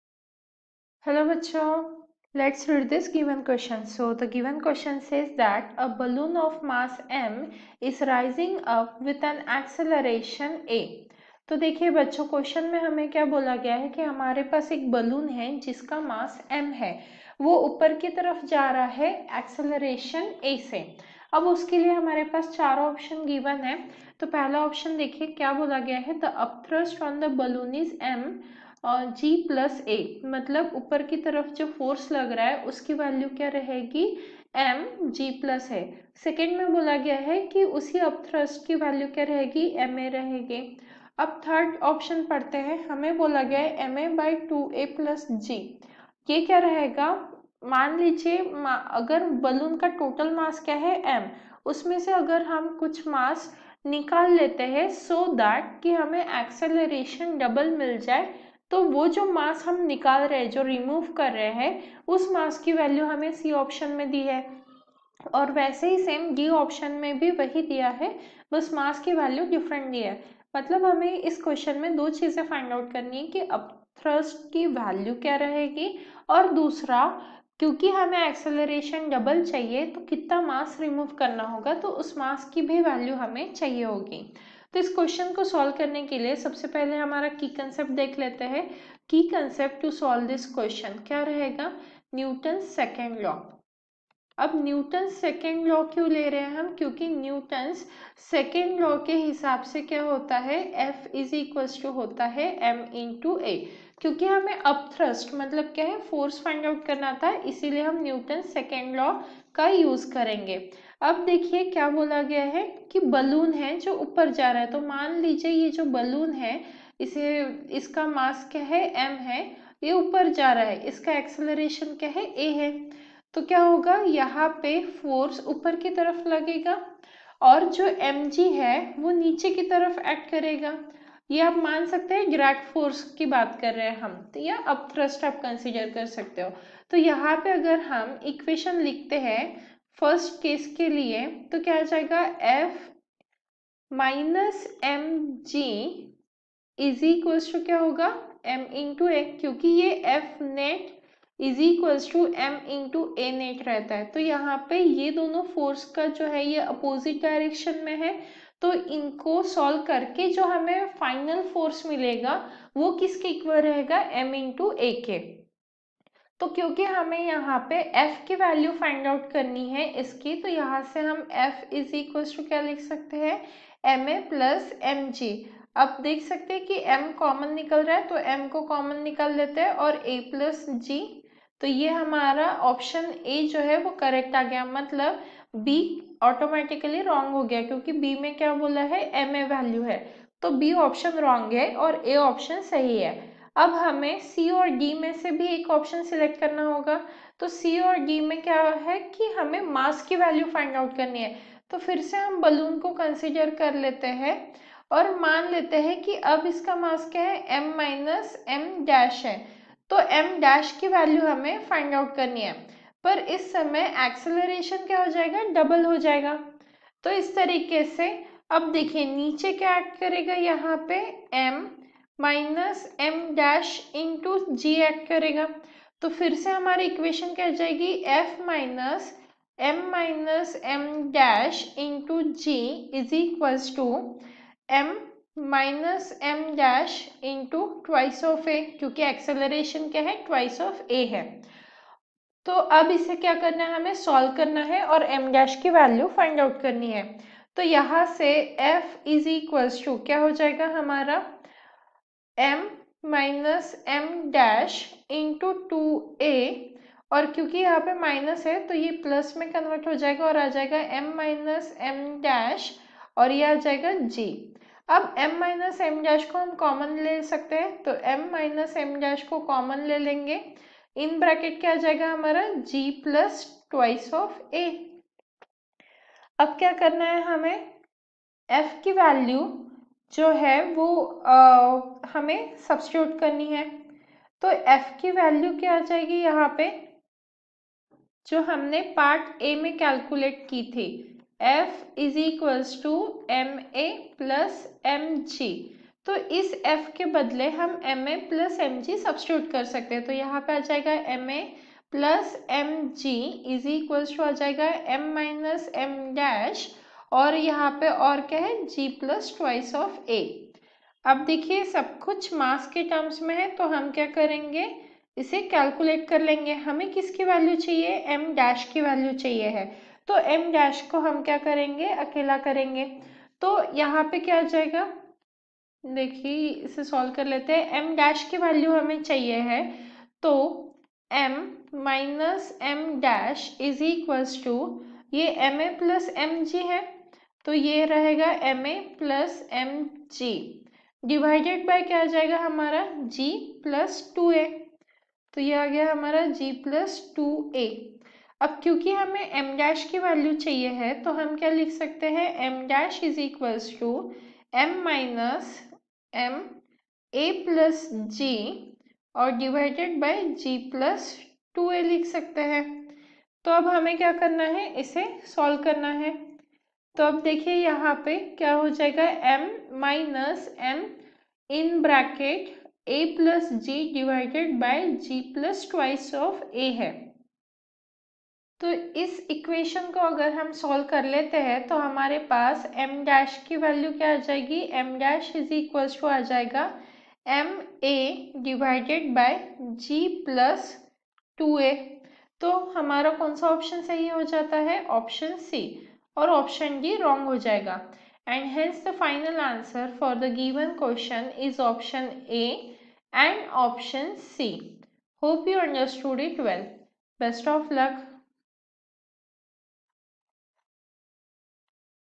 A balloon of mass \( m \) is rising up with an acceleration \( a \). (A) The upthrust on the bal...
A balloon of mass \( m \) is rising up with an acceleration \( a \).
(A) The upthrust on the balloon is \( m(g+a) \).
(B) The upthrust on the balloon is \( m a \).
(C) \( m a /(2 a+g) \) mass must be detached in order to double its acceleration.
(D) \( \mathrm{m} / 2 \) mass must be detached in order to double its acceleration.